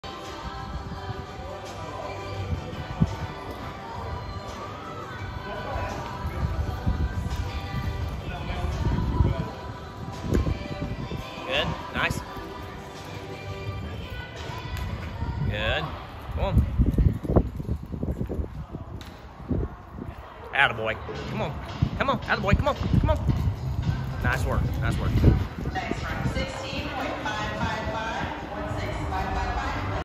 BQH SB Blazin Glory and Collin Allen
2017 aqha bay mare by Blazin Jetolena out of Gold Astreaken by Fols Gold.